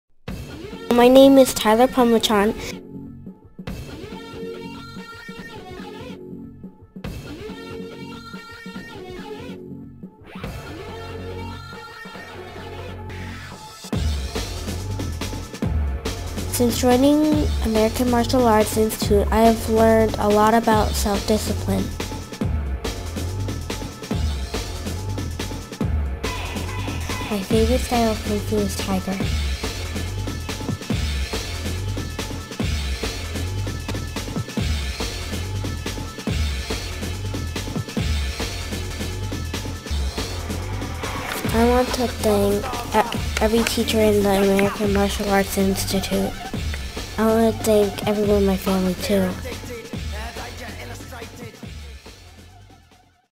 08. Tyler Phommachanh.avi
Premiered March 6th, 2010, AMAI's 7th Annual Graduation Banquet, Dragon Black Sash Video highlight - Tyler Phommachanh. Dragon Black Sash is the Black Sash for kids ages 7-11. Jr. Black Sash is for teens 12-15. Student eligable for the Adult Black Sash must be at least 16 years old or 15 and progressed through the Dragon and or Jr Black Sash ranks.